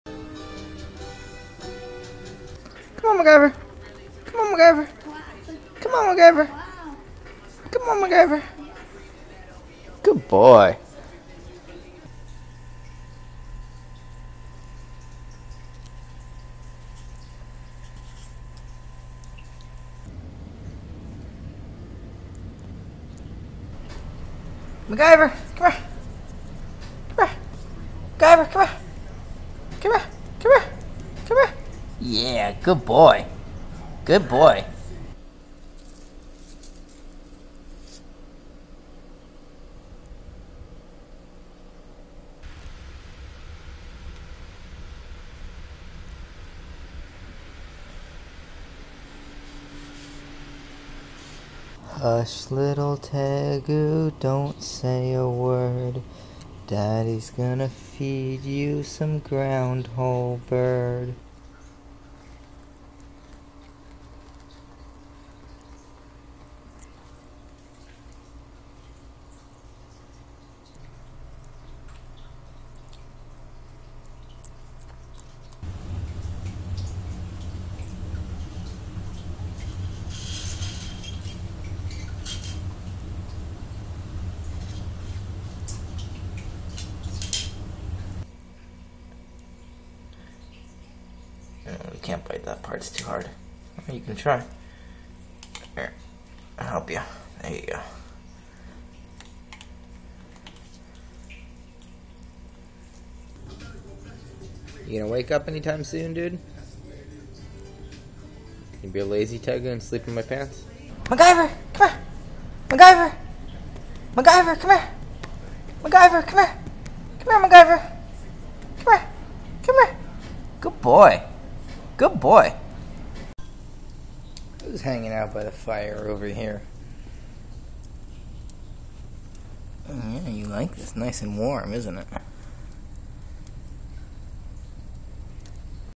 ☜ MacGyver the Tegu Lizard
Mac is just a baby in these clips, so make sure to check out more recent videos to see him as 4.5 feet long and 19 pounds.